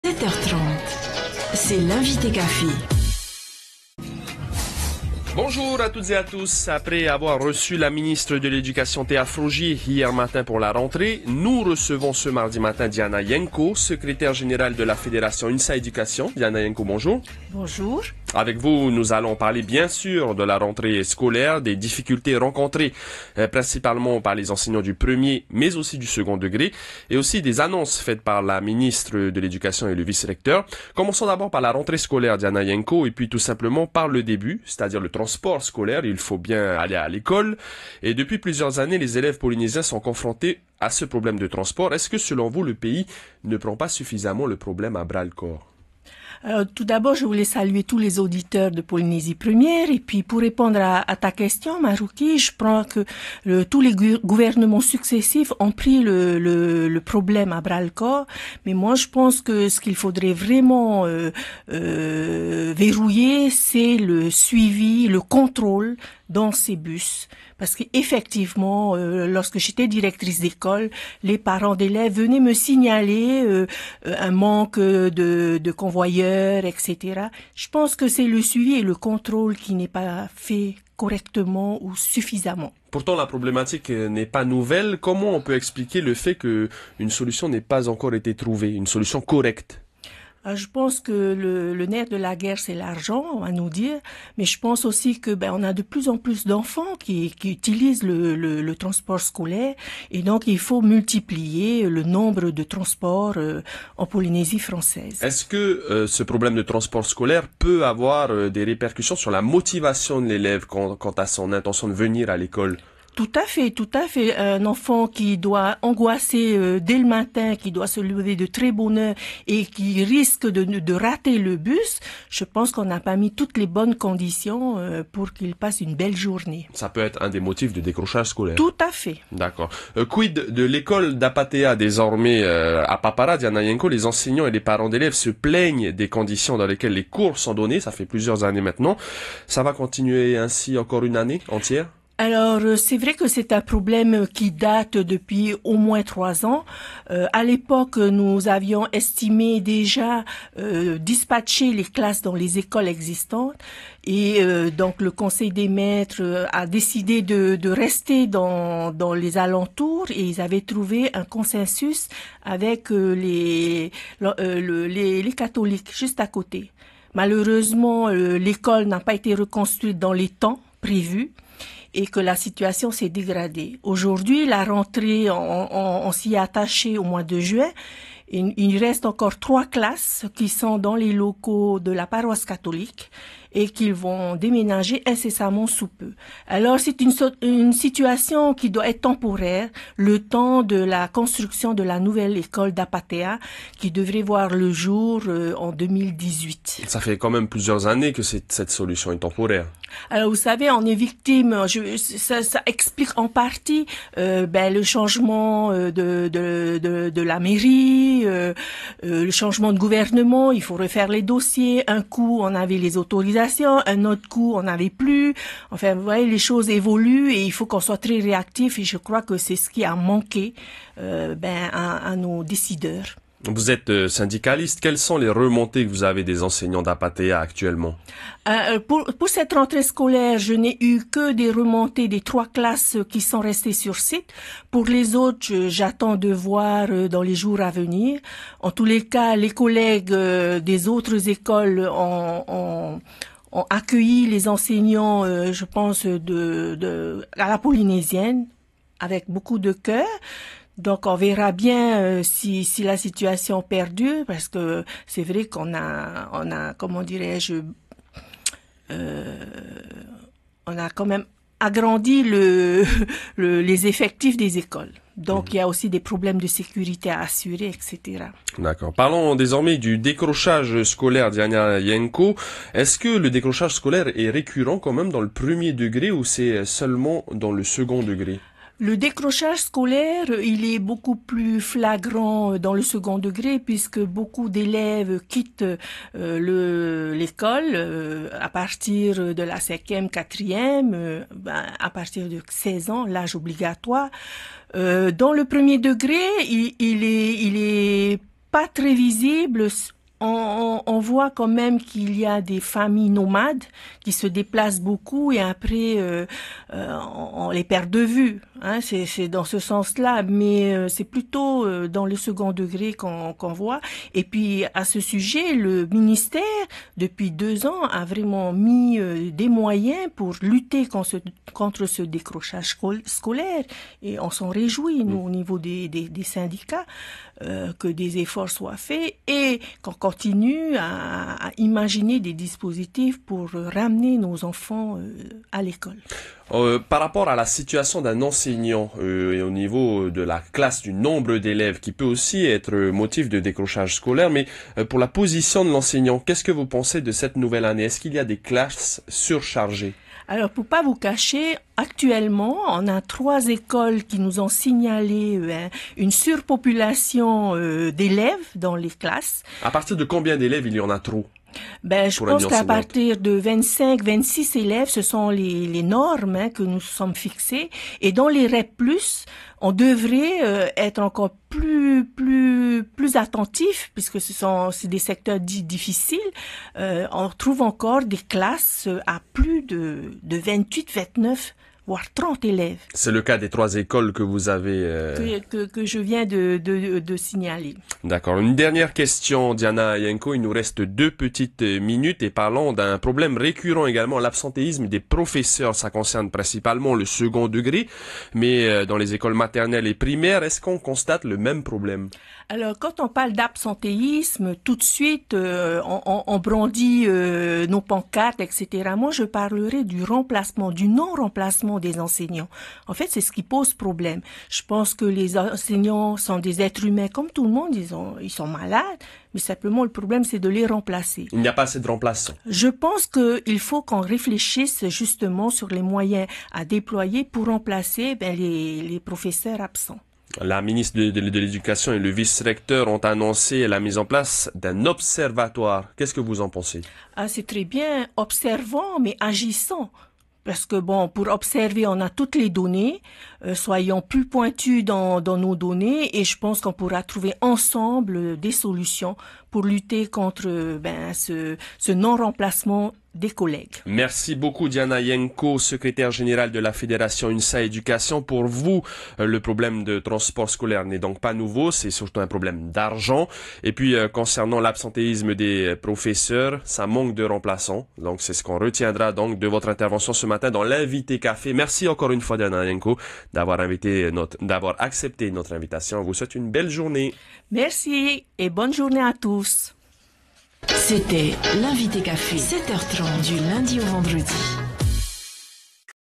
Invitée café : Diana Yieng Kow - 22/08/2017
L'invitée café du mardi 22 août 2017 est Diana Yieng Kow, secrétaire générale de la fédération syndicale UNSA Education. Elle est intérrogée par Maruki Dury sur différentes questions autour de transports  scolaires.